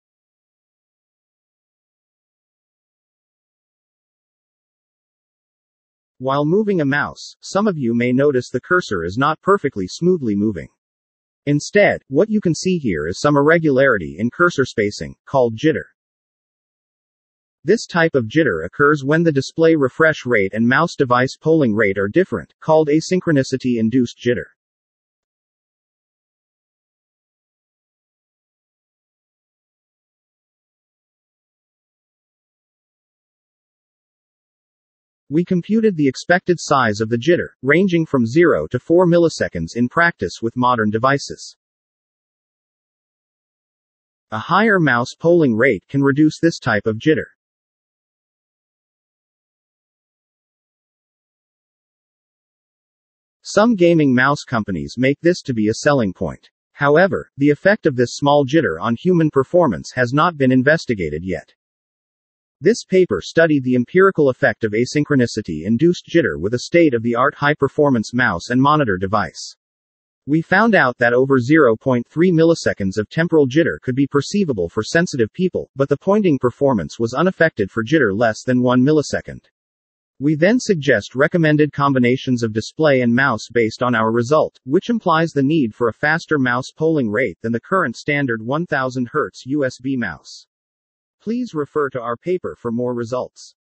Do We Need a Faster Mouse? Empirical Evaluation of Asynchronicity-Induced Jitter
Auejin Ham, Junsu Lim, Sunjun Kim

Session: Typing and Pointing

Abstract
In gaming, accurately rendering input signals on a display is crucial, both spatially and temporally. However, the asynchronicity between the input and output signal frequencies results in unstable responses called "jitter." A recent research modeled this jitter mathematically; however, the effect of jitter on human performance is unknown. In this study, we investigated the empirical effect of asynchronicity-induced jitter using a state-of-the-art high-performance mouse and monitor device. In the first part, perceptual user experience under different jitter levels was examined using the ISO 4120:2004 triangle test protocol, and a jitter of over 0.3 ms could be perceived by sensitive subjects. In the second part, we measured the pointing task performance for different jitter levels using the ISO 9241-9 (i.e., Fitts' law) test, and found that the pointing performance was unaffected up to a jitter of 1 ms. Finally, we recommended display and mouse combinations based on our results, which indicated the need for a higher mouse polling rate than that of the current standard 1000-Hz USB mouse.

Full Videos of the UIST 2021 Papers Program